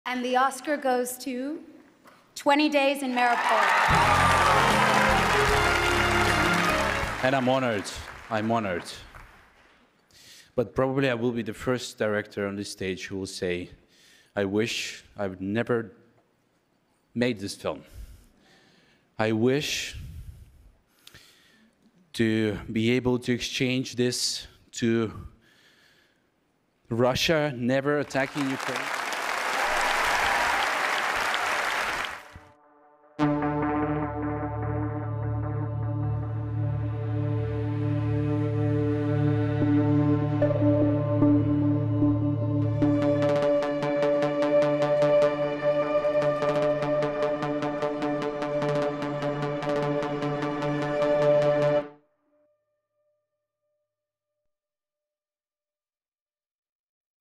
Congratulations to 20 Days in Mariupol for winning the Oscar for Best Documentary.
TruthJusticeReparations for victims of Russian aggression in Ukraine.